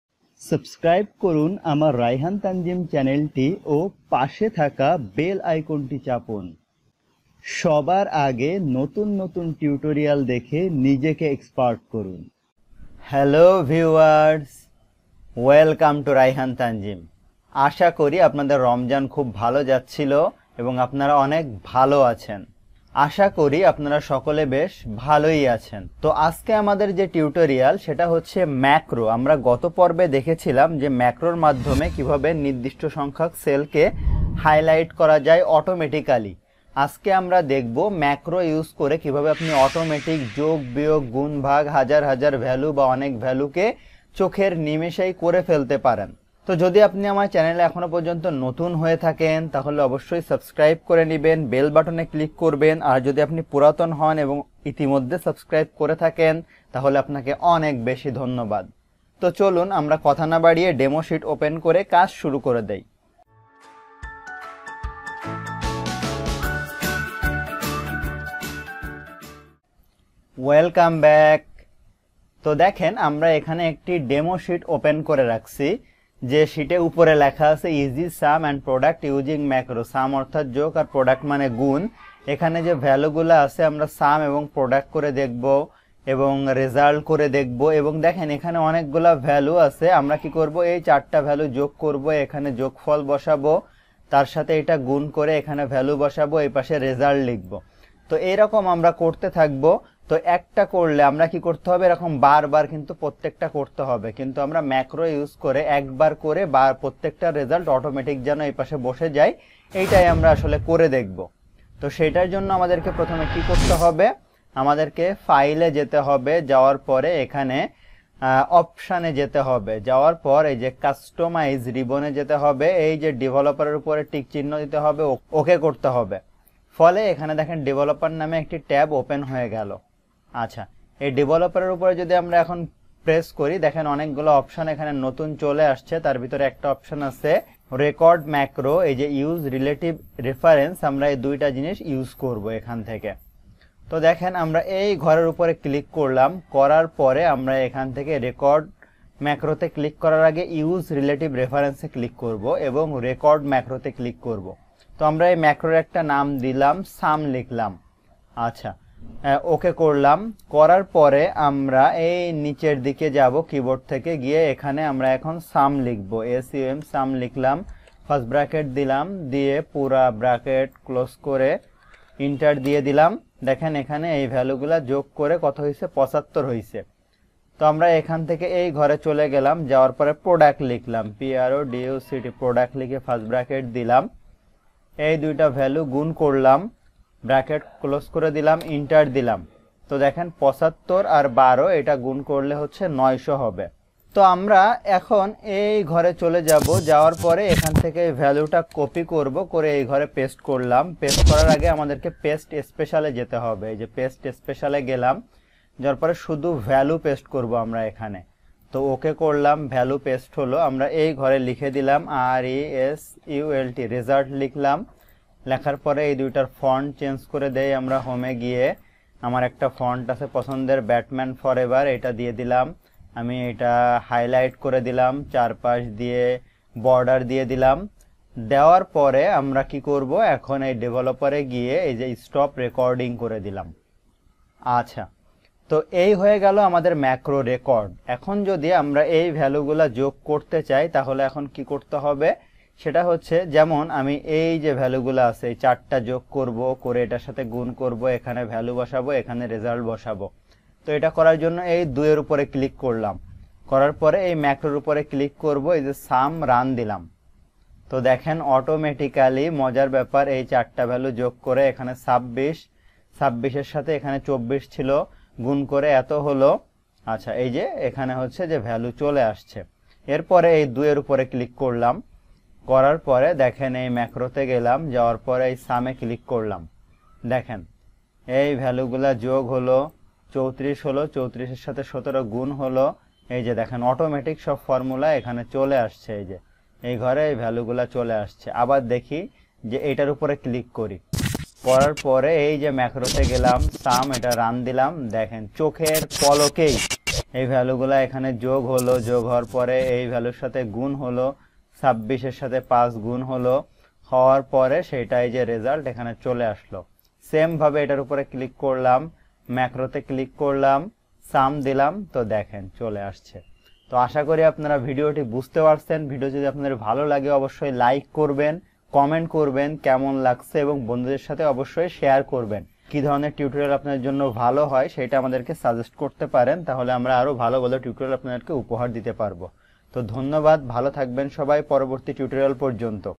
How to Create a Simple Macro in Excel || Part-2 || MS Excel Tutorial Bangla
In this tutorial, I will try to show how to create macros in excel 2010 || Excel Bangla tutorial. And how to work with macros in excel 2010, and what the necessity of macro in excel. You will be able to learn how to use excel macro training in Bangla and what is macro in excel. How do summation multiplication subtraction and division using macro work properly? And what are MS excel tutorial and excel macro recorder actually? After watching this video you will be an expert in Macro for beginners in Bangla 2010 by rayhan tanjim using Bangla language.